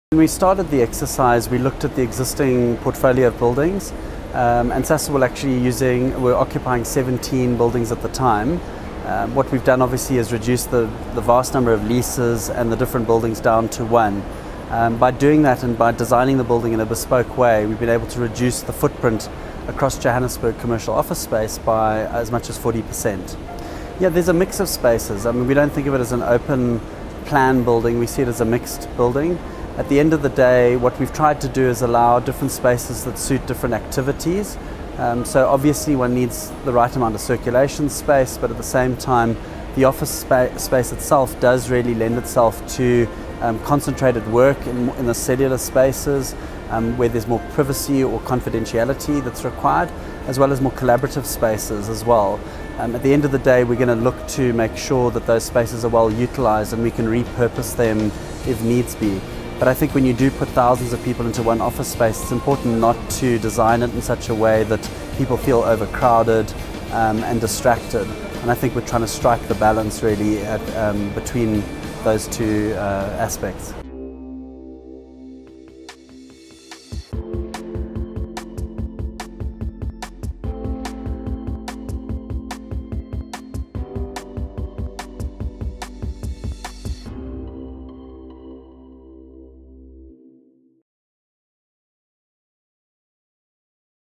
Sasol shows off new corporate head office
The final phase of global chemicals company Sasol’s new headquarters, Sasol Place, in Sandton, its tenth floor, was commissioned on Monday. This not only houses its group executive suites, but also hosts the company’s engagement facilities.

This completes the 11-storey, 47-m-tall office complex, which holds 67 000 m2 of rentable office area. Speaking during a media tour on Tuesday, real estate management company Betapoint CEO and Sasol tenant representative and project management and integration lead Adam Sargent explained that, while the new building was massive, it reduced Sasol’s office footprint by up to almost 40%.